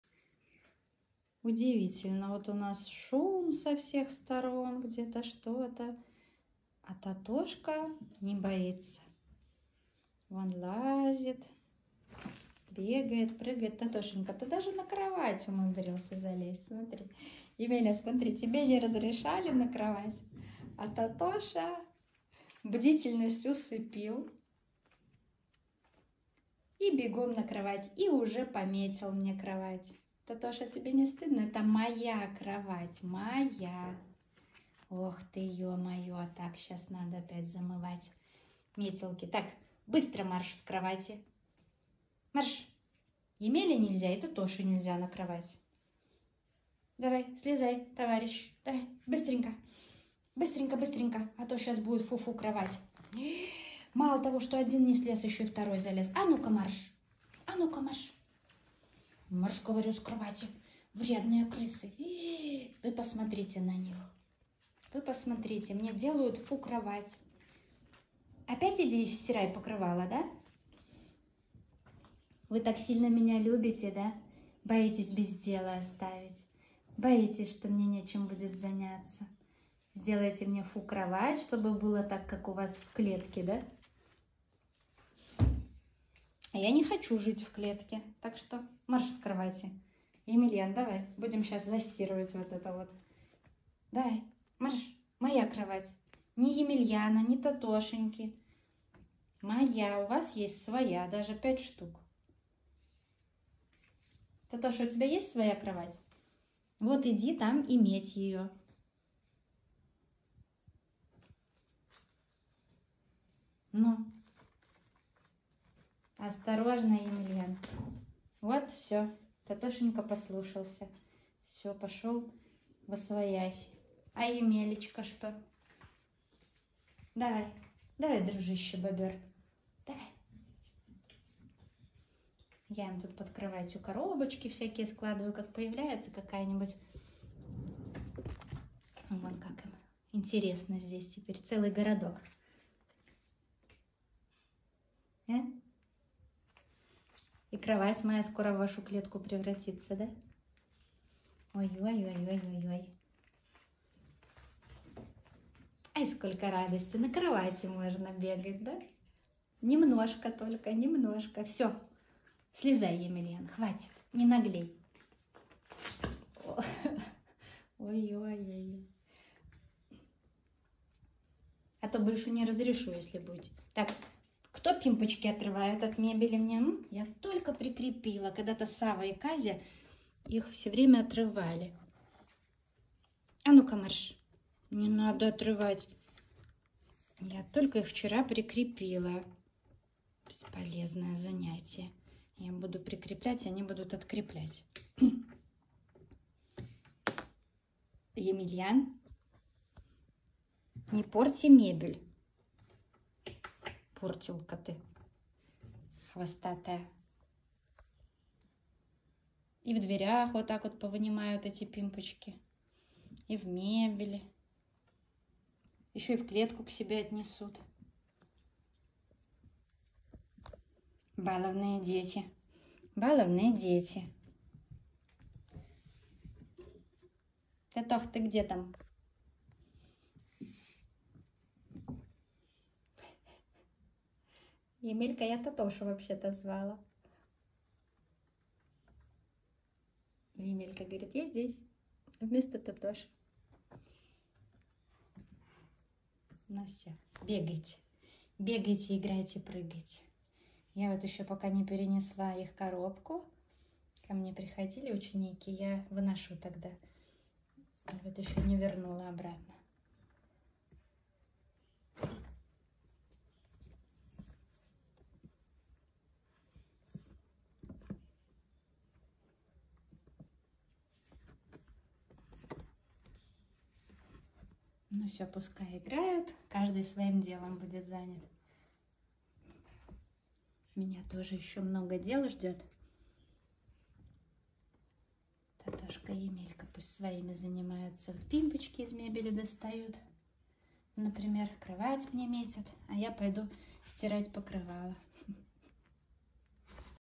Хитрый крыс Татоша.
Татоша сегодня в ударе! На него это совсем не похоже! Посмотрите какой он активный! Прям удивительно! Не подменил ли нам кто Татошу? 😂😂😂 Крысы это весьма забавные животные и непредсказуемые. Это делает общение с ними особенно интересным.
Только посмотрите на этих красавчиков 😍🐀🐀🥰❤️